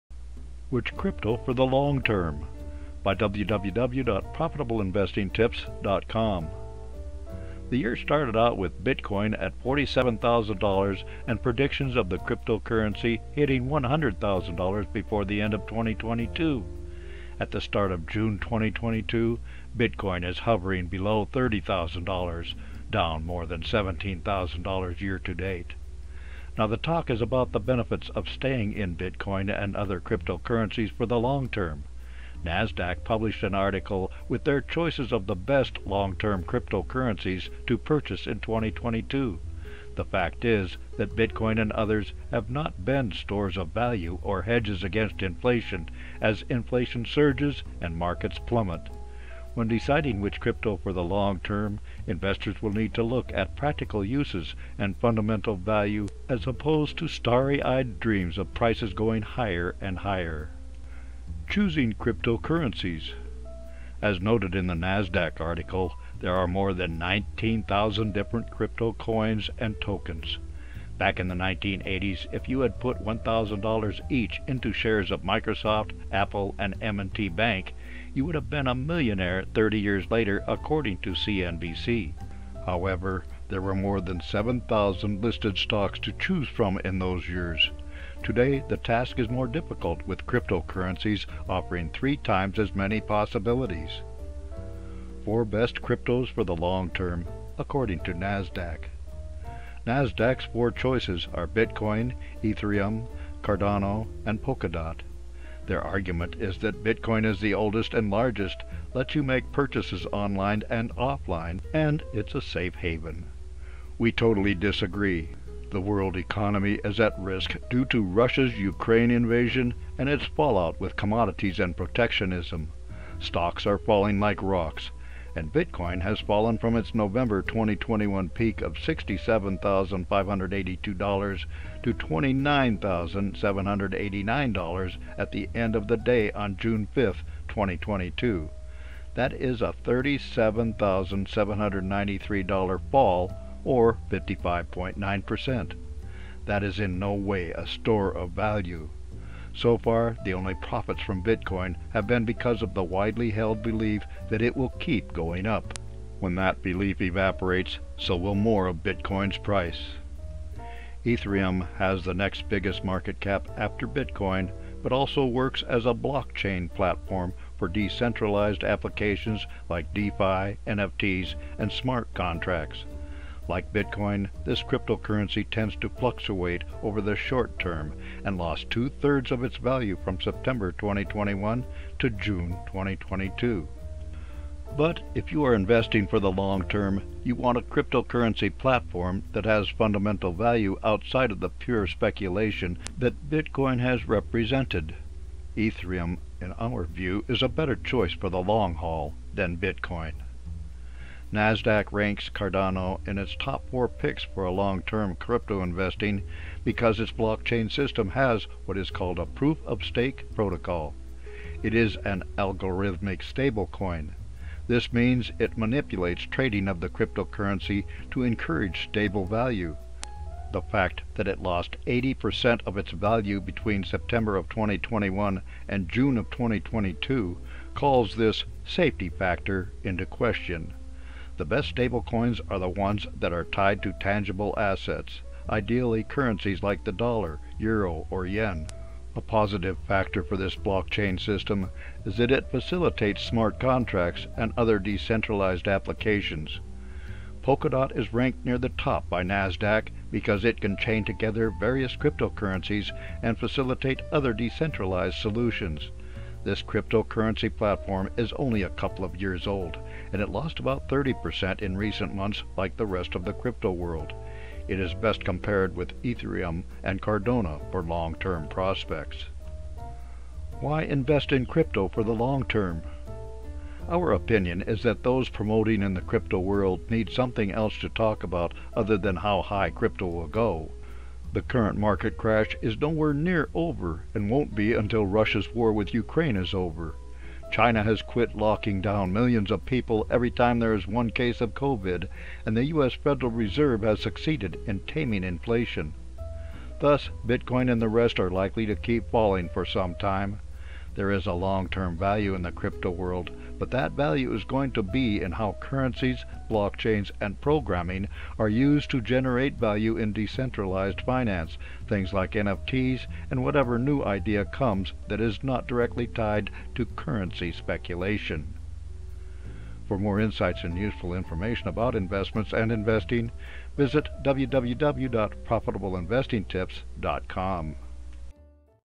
Which Crypto for the Long Term?
When deciding which crypto for the long term investors will need to look at practical uses and fundamental value as opposed to stary-eyed dreams of prices going higher and higher.

The year started out with Bitcoin at $47,000 and predictions of the cryptocurrency hitting $100,000 before the end of 2022. At the start of June 2022 Bitcoin is hovering below $30,000 (down more than $17,000 YTD). Now the talk is about the benefits of staying in Bitcoin and other cryptocurrencies for the long term. Nasdaq published an article with their choices of the best long-term cryptocurrencies to purchase in 2022. The fact is that Bitcoin and others have not been stores of value or hedges against inflation as inflation surges and markets plummet. When deciding which crypto for the long term investors will need to look at practical uses and fundamental value as opposed to stary-eyed dreams of prices going higher and higher.
Choosing Crypto Currencies
As noted in the Nasdaq article, there are more than 19,000 different crypto coins and tokens! Back in the 1980s if you had put $1,000 each into shares of Microsoft, Apple, and M&T Bank you would have been a millionaire thirty years later according to CNBC. However, there were more than 7,000 listed stocks to choose from in those years. Today the task is more difficult with cryptocurrencies offering three times as many possibilities.
Four Best Cryptos for the Long Term (According to Nasdaq)
Nasdaq’s four choices are Bitcoin, Ethereum, Cardano, and Polkadot. Their argument is that Bitcoin is the oldest and largest, lets you make purchases online and offline, and it a safe haven. We totally disagree. The world economy is at risk due to Russia’s Ukraine invasion and its fallout with commodities and protectionism. Stocks are falling like rocks. And, Bitcoin has fallen from its November 2021 peak of $67,582 to $29,789 at the end the day on June 5, 2022. That is a $37,793 fall or 55.9%. That is in no way a store of value. So far, the only profits from Bitcoin have been because of the widely held belief that it will keep going up. When that belief evaporates so will more of Bitcoin’s price.
Ethereum has the next biggest market cap after Bitcoin but also works as a blockchain platform for decentralized applications like DeFi, NFTs, and smart contracts. Like Bitcoin this cryptocurrency tends to fluctuate over the short term and lost two-thirds of its value from September, 2021 to June 2022.
But if you are investing for the long term, you want a cryptocurrency platform that has fundamental value outside of the pure speculation that Bitcoin has represented. Ethereum, in our view, is a better choice for the long haul than Bitcoin.
Nasdaq ranks Cardano in its top four picks for long term crypto investing because its blockchain system has what is called a proof-of-stake protocol. It is an algorithmic stablecoin. This means it manipulates trading of the cryptocurrency to encourage stable value. The fact that it lost 80% of its value between September of 2021 and June of 2022 calls this “safety factor” into question. The best stablecoins are the ones that are tied to tangible assets, ideally currencies like the dollar, euro, or yen. A positive factor for this blockchain system is that it facilitates smart contracts and other decentralized applications.
Polkadot is ranked near the top by Nasdaq because it can chain together various cryptocurrencies and facilitate other decentralized solutions. This cryptocurrency platform is only a couple of years old and it lost about 30% in recent months like the rest of the crypto world. It is best compared with Ethereum and Cardona for long term prospects.
Why Invest in Crypto for the Long Term?
Our opinion is that those promoting in the crypto world need something else to talk about other than how high crypto will go. The current market crash is nowhere near over and won’t be until Russia’s war with Ukraine is over, China has quit locking down millions of people every time there is one case of Covid, and the US Federal Reserve has succeeded in taming inflation. Thus, Bitcoin and the rest are likely to keep falling for some time. There is long term value in the crypto world but that value is going to be in how currencies, blockchains, and programming are used to generate value in decentralized finance, things like NFTs, and whatever new idea comes that is not directly tied to currency speculation.
For more insights and useful information about investments and investing,